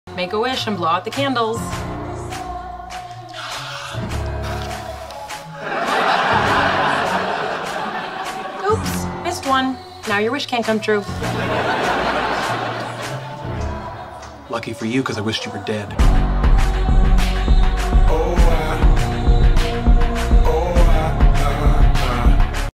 “I Don’t Need To Celebrate With You.” Meme gc [trend]
Vincent really dislikes his own father

This is pretty much the reason why he doesn’t celebrate when it comes to his birthday

Thanks